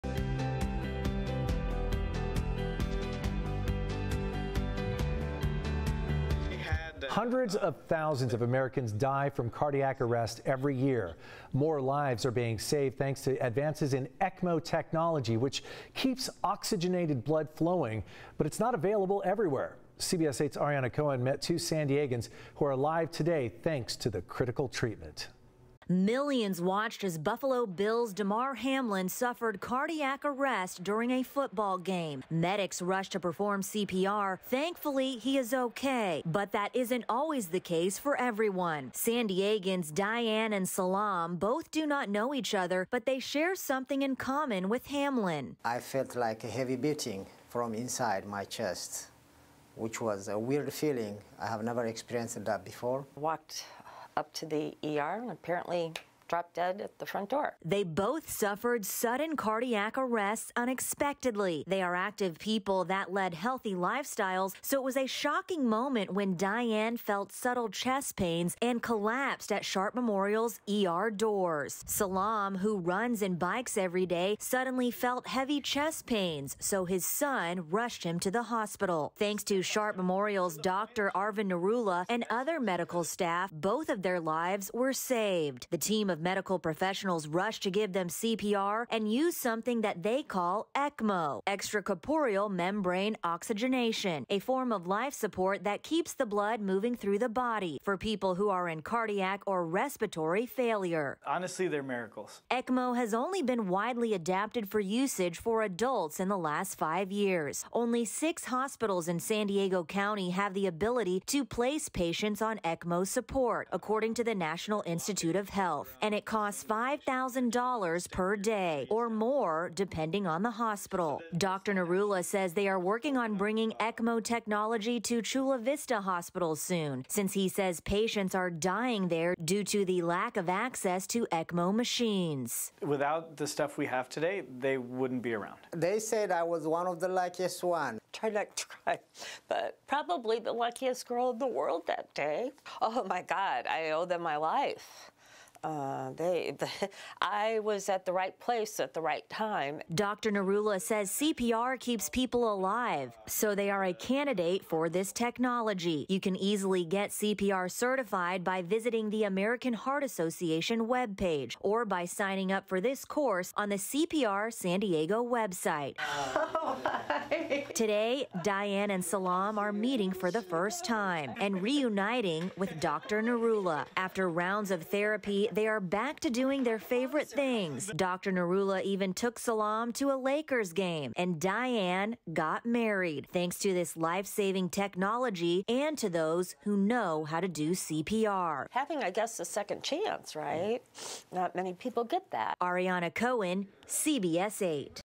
Cardiac arrest survivors are saved with ECMO technology | Here's how ECMO works
Millions watched as Buffalo Bills’ Damar Hamlin suffered cardiac arrest during a football game. Medics rushed to perform CPR. He is on the path to recovery, but that isn’t always the case for everyone.

San Diegans Diane McGrogan and Selam Tesfalidet do not know each other, but they share something in common with Hamlin.

"I heard heavy beating in my chest. I've never heard that before," said Tesfalidet.

"I walked up to the ER and dropped dead at the front door," said McGrogan.

They both suffered sudden cardiac arrests unexpectedly.

They are active people that lead healthy lifestyles, so it was a shocking moment when McGrogan felt subtle chest pains and collapsed at the Sharp Memorial’s ER doors.

Tesfalidet, who runs and bikes everyday, suddenly felt heavy chest pains, so his son rushed him to the hospital.

Thanks to Sharp Memorial’s Dr. Arvin Narula and other medical staff, both of their lives were saved.